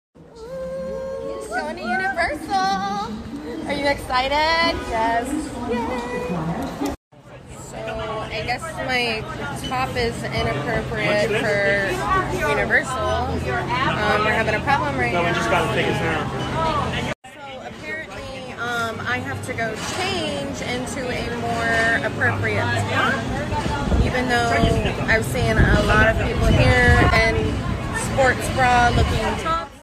Model claims she was told to cover up breasts at Universal Studios
A model has claimed she was 'sexualized' and 'discriminated' against by Universal Studios employees after she was banned from entering the theme park because she was wearing a skimpy crop top that showed off her cleavage.
Whitney Paige Venable, 26, from Florida, says she was 'humiliated and embarrassed' after being barred from the Orlando amusement park - revealing that staff told her she would not be able to gain access until she had covered up her chest in a T-shirt.
Sharing footage from the incident, which took place in June 2021 but has only just been brought to light on social media, Whitney explained that she turned up at Universal wearing a white sports bra and mint green shorts - while claiming that many other women were wearing similar outfits.
She said her clothing 'wasn't against the rules' and blasted the park for 'targeting' her for the 'size of her breasts.'
The model said her 'naturally big breasts' often 'make people uncomfortable' - but insisted that she is sick of being treated differently than 'smaller women.'
I felt humiliated and embarrassed,' Whitney, who has more than one million followers on Instagram, told Jam Press. 'I was told I had to change my top to be able to enter.'
'I think it's because I have naturally big breasts and it makes people uncomfortable but at far as I know my clothing wasn't against the rules.
'I feel like I was discriminated against because of the size of my breasts. I noticed other people staring and I felt really embarrassed. It was humiliating.'
In a video shared with Jam Press, Whitney could be seen on her way into Universal Studios with a friend, with the pair looking excited for their day out.
Moments later, their happy mood was crushed as a second video showed them standing at the gate of the park, being told that they couldn't come in.
'So, I guess my top is inappropriate for Universal, we're having a problem right now,' she could be heard saying, before panning the camera towards a few of the staff members.
In a follow-up video, Whitney, who was ultimately turned away at the entrance, said: 'So apparently I have to go change into a more appropriate top even though I've seen a lot of people here in sports bra-looking tops.'